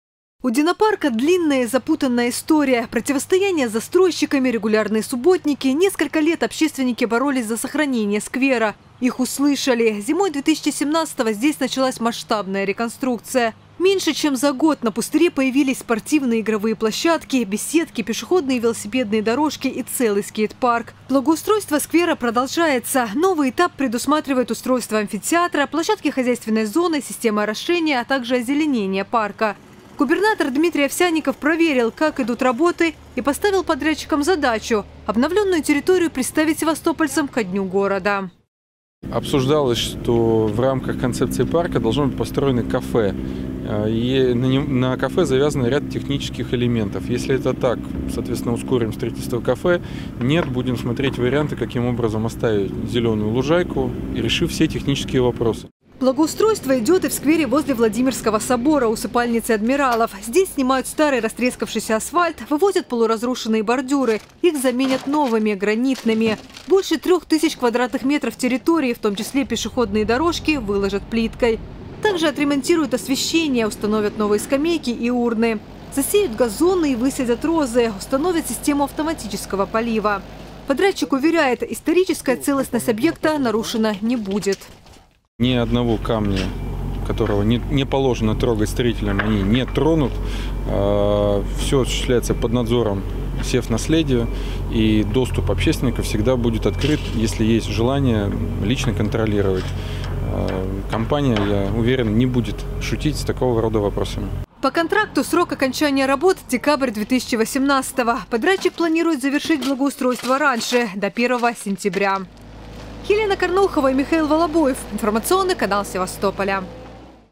Благоустройство Севастополя — под контролем губернатора
Стартовал второй этап работ по благоустройству и в Динопарка. У Рыбацкого сквера, а в народе — Динопарка, — длинная и запутанная история. Противостояние с застройщиками, регулярные субботники — несколько лет общественники боролись за сохранение сквера. Их услышали: зимой 2017-го здесь началась масштабная реконструкция. Меньше чем за год на пустыре появились спортивные и игровые площадки, беседки, пешеходные и велосипедные дорожки и целый скейт-парк. Благоустройство сквера продолжается. Новый этап предусматривает устройство амфитеатра, площадки хозяйственной зоны, системы орошения, а также озеленение парка. Губернатор Дмитрий Овсянников проверил, как идут работы и поставил подрядчикам задачу: обновленную территорию представить севастопольцам ко Дню города, 14-го июня.